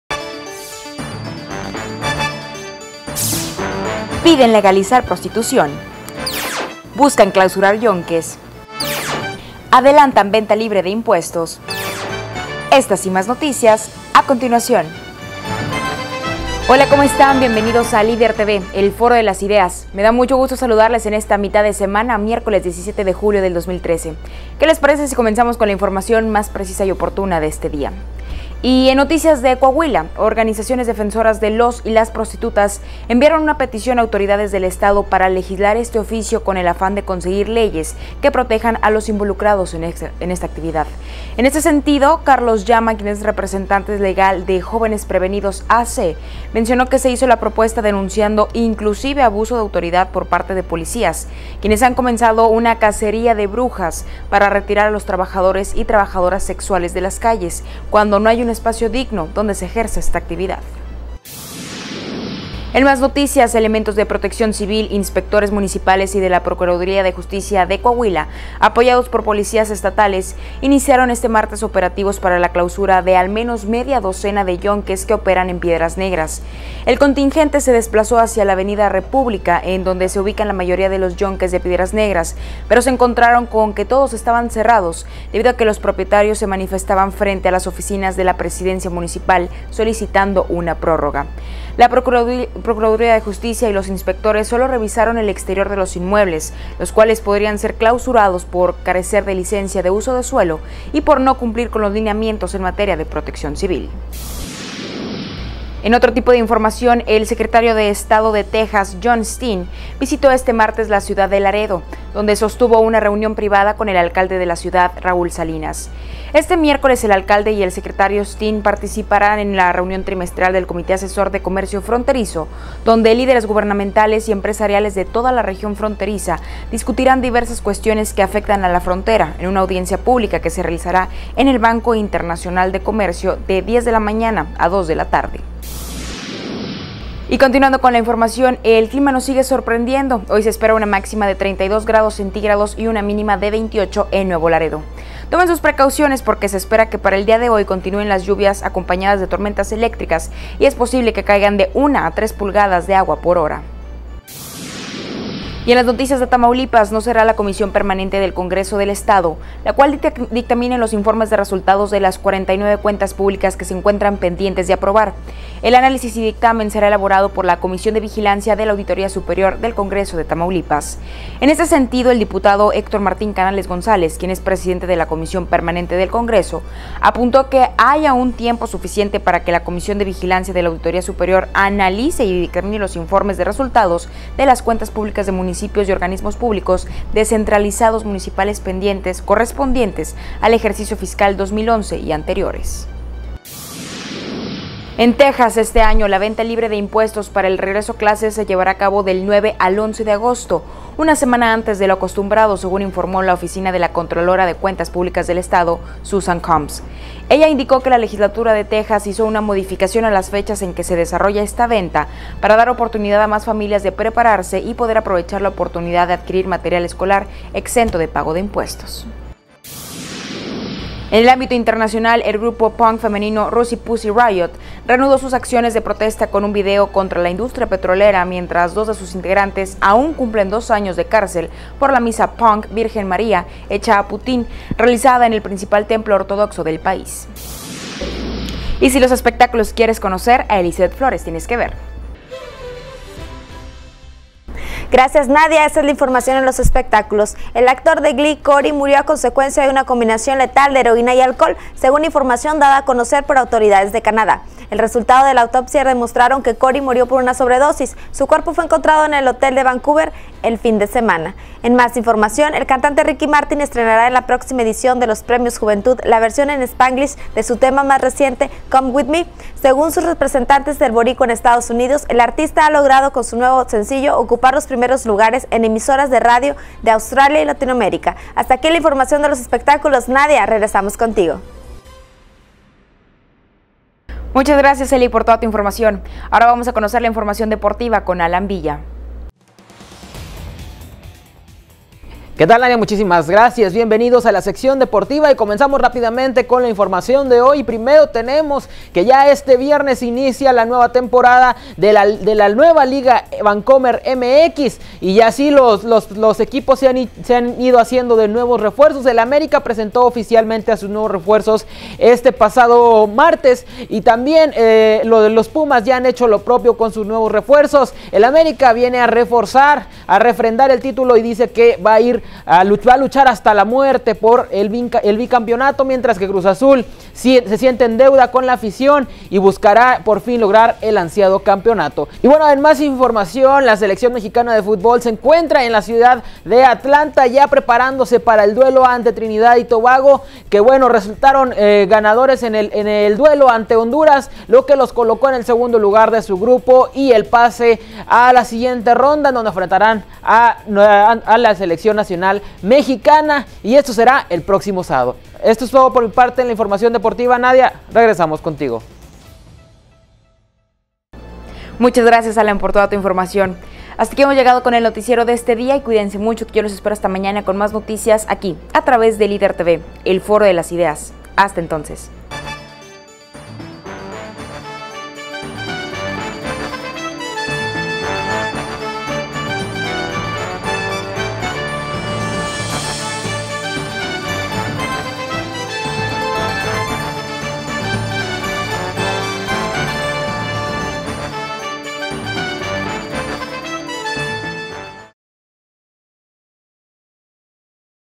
Piden legalizar prostitución / Secretario de Texas visita Laredo
Piden legalizar prostitución
Buscan clausurar yonques
Secretario de Texas visita Laredo
Adelantan venta libre de impuestos
Comisión Permanente no revisará cuentas públicas
Reaparece Pussy Riot contra industria petrolera
"Mala combinación" le costo la vida a Cory Monteith
Ricky Martin estrenará sencillo próximamente
Se acerca inicio de temporada
México se prepara para Trinidad y Tobago